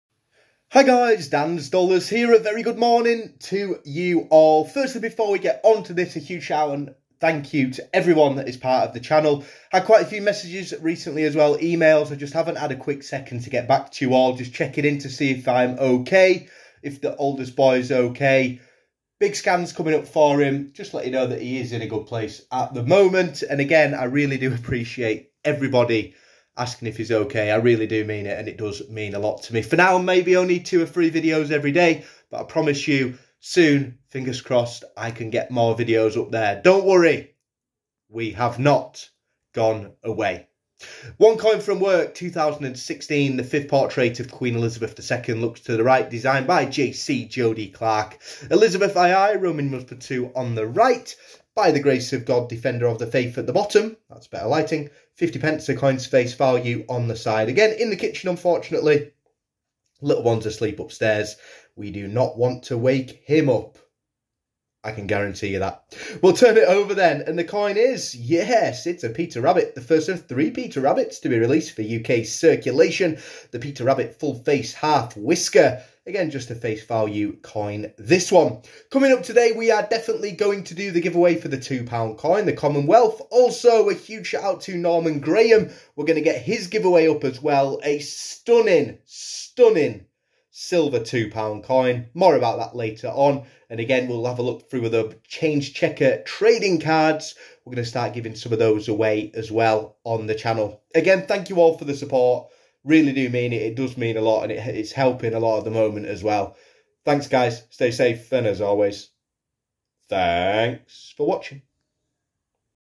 2016 50p coin find! CHECK YOUR CHANGE #1062
UK 2016 50p coin find! CHECK YOUR CHANGE #1062 - Over 1000 COIN FINDS WITH YOU. King charles iii 50p and Queen Elizabeth II 50 pence coin find. Best of luck with all of your coin hunts as we continue the coin hunting journey on the channel